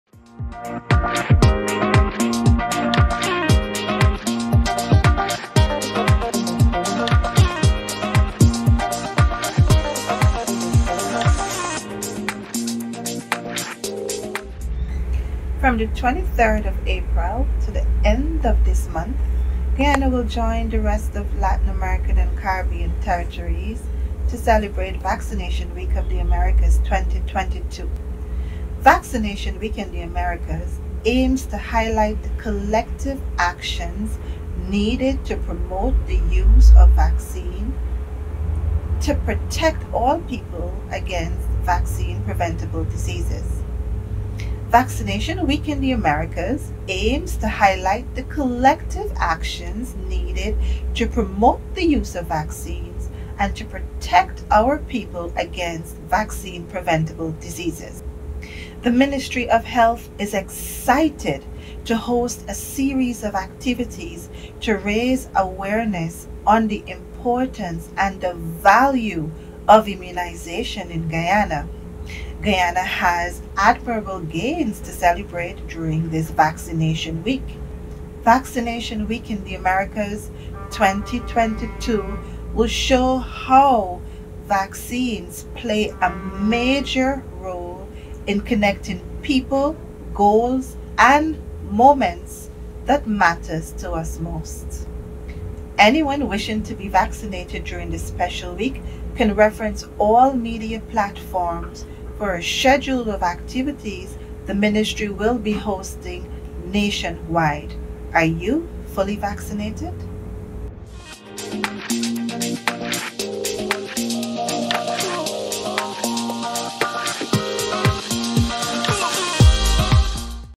Dr Scott vaccination message
Vaccination Week in the Americas 2022
Message from Dr Oneka Scott, Maternal and Child Officer attached to the Ministry of Health.